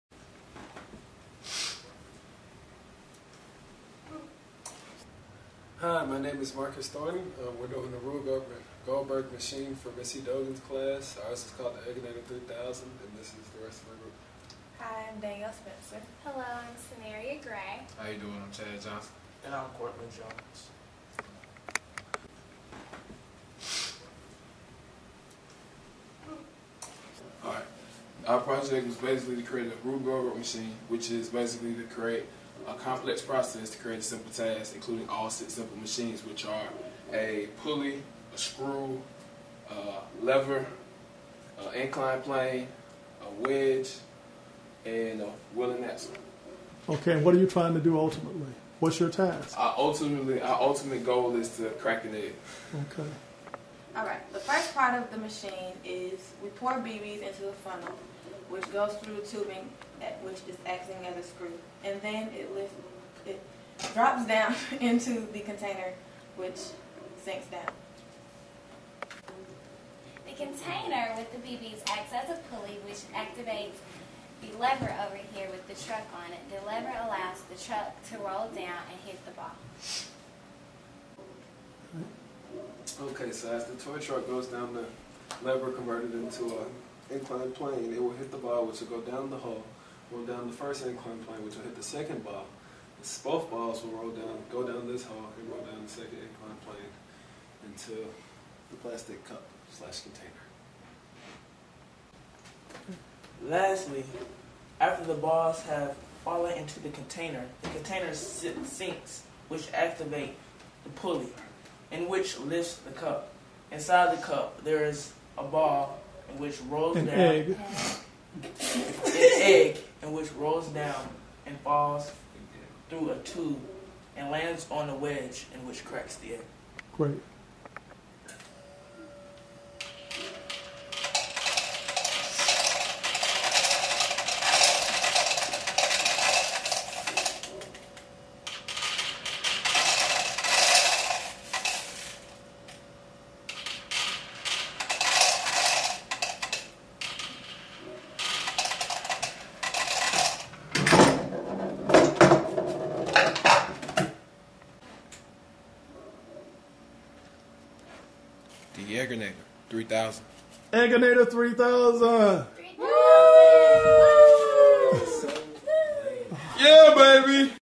Egginator 3000 Rube Goldberg Machine
Rube Goldberg Machine to crack an egg.  Westlake HS, Atlanta, GA.  By Marcus Thornton, Chad Johnson, Danielle Spencer, S'Maria Gray & Kourtland Jones for Ms. Modupe Edogun, November 2008.